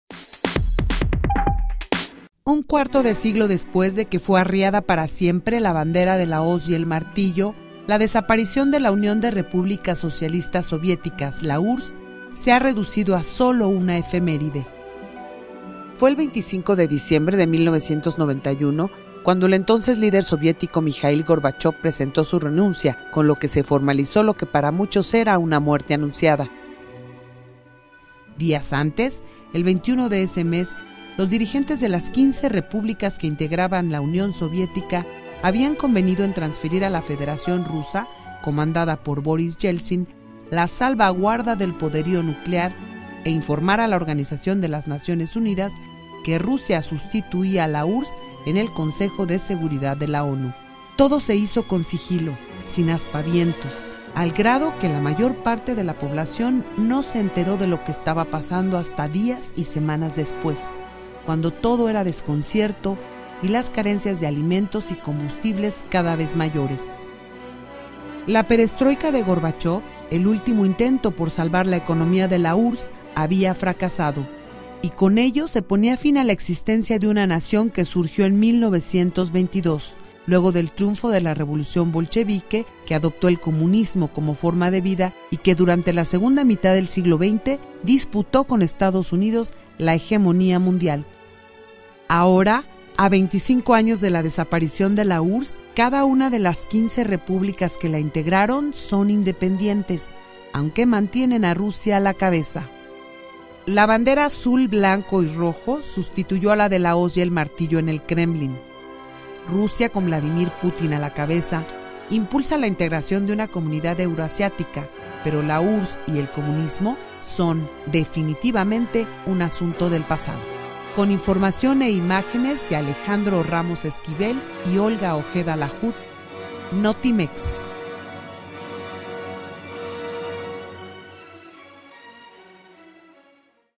La URSS, 25 años del adiós
Fue el 25 de diciembre de 1991 cuando el entonces líder soviético, Mijail Gorbachov, presentó su renuncia, con la que se formalizó, lo que para muchos era una muerte anunciada...